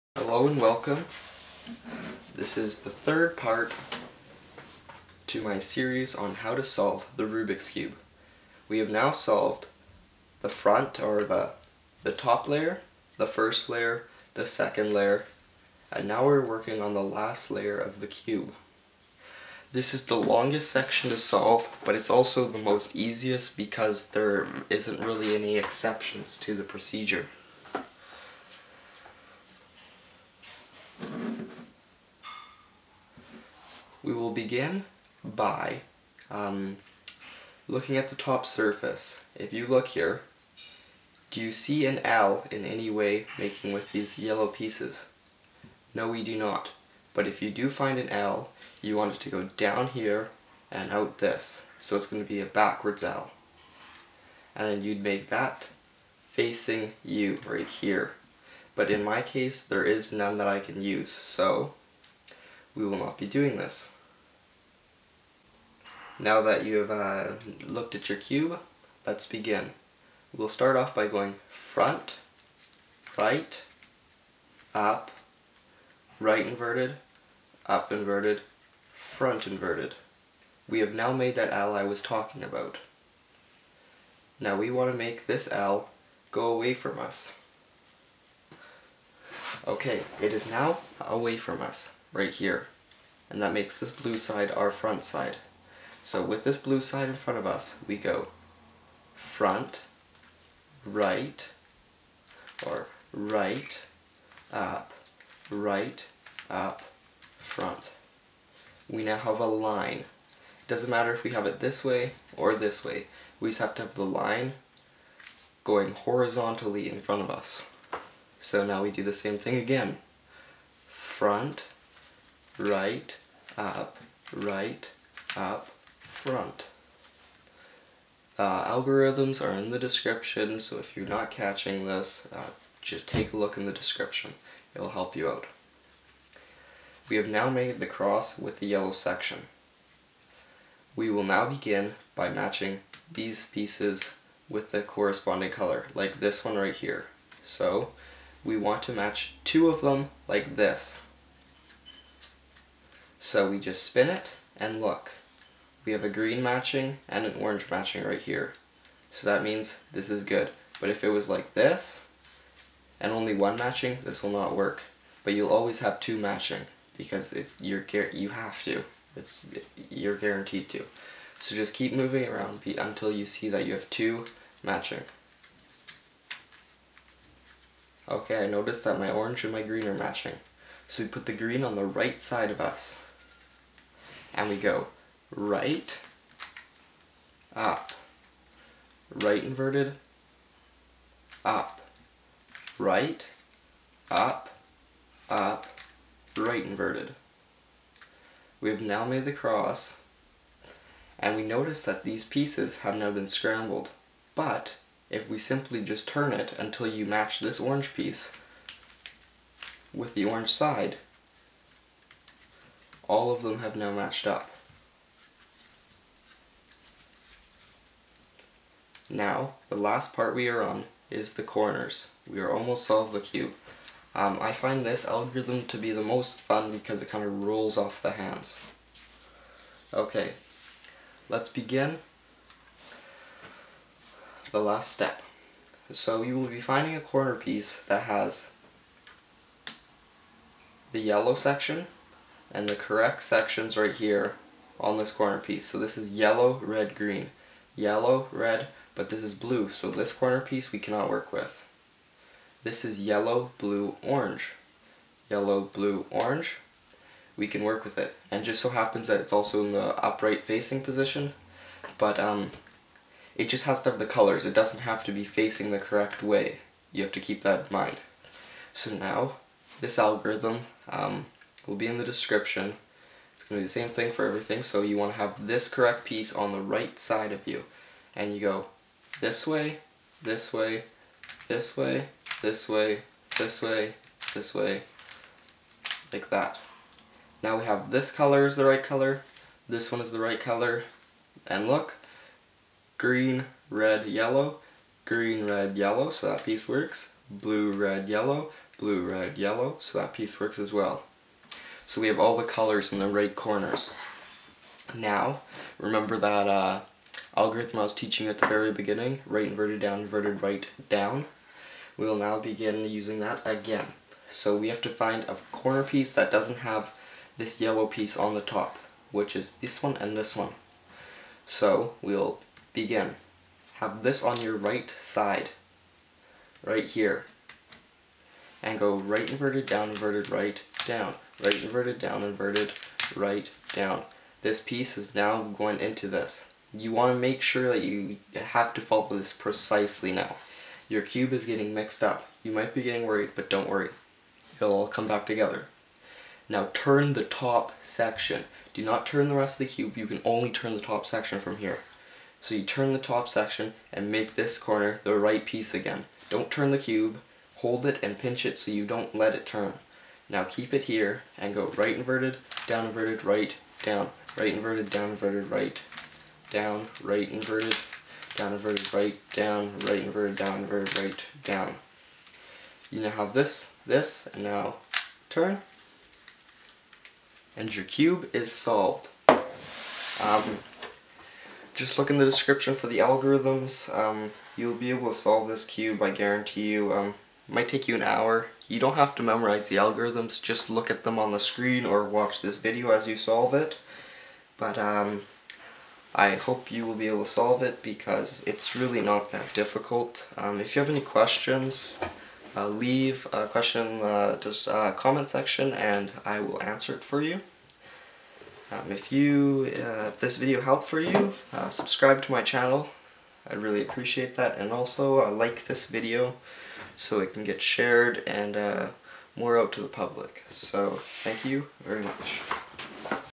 How To Solve A Rubik's Cube - Part 3
In this video, I show you how to easily solve the original Rubik's Cube Puzzle.

ALGORITHMS

***First Layer***
Flip Edge: F' U L' U'
Corners
R' D' R D

***Middle Layer***
Edge Belongs on the Left: U' L' U L U F U' F'
Edge Belongs on the Right: U R U' R' U' F' U F

***Last Layer***
Getting the Cross
From Dot: (Hold dot in any position): F R U R' U' F'
From L (Hold L in top left): F R U R' U' F
From Bar (Hold bar horizontally): F R U R' U' F'

Positioning the Cross
(Hold so one of the colors is to your right and the other is in the back):
R U R' U R 2U R' U

Positioning Corners
If NO corners are positioned: U R U' L' U R' U' L
Hold the solved corner in the top right: U R U' L' U R' U' L
(Repeat until all four corners are positioned correctly)

Orienting Corners (FINAL STEP!!!)
Hold unsolved corners in the top right corner: R' D' R D
(Repeat until all four corners are oriented correctly)
Only spin the top layer